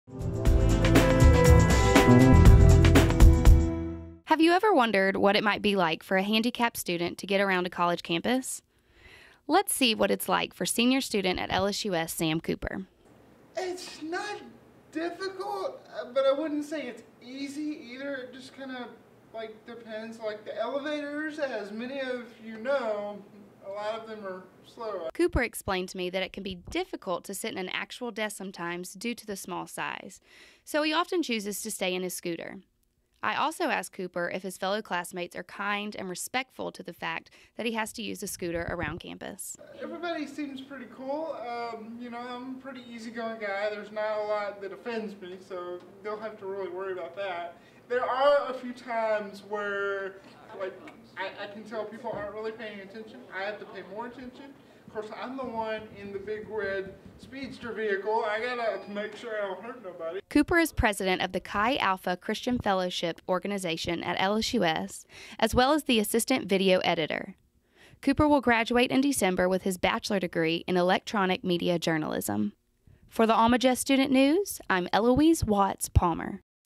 A Day in the Life of Sam Cooper
Ever wonder what it's like to be a student at LSUS with a disability? Reporter Eloise Watts Palmer follows the Almagest's own Sam Cooper to get his point of view!
Keep up to date on all your LSUS News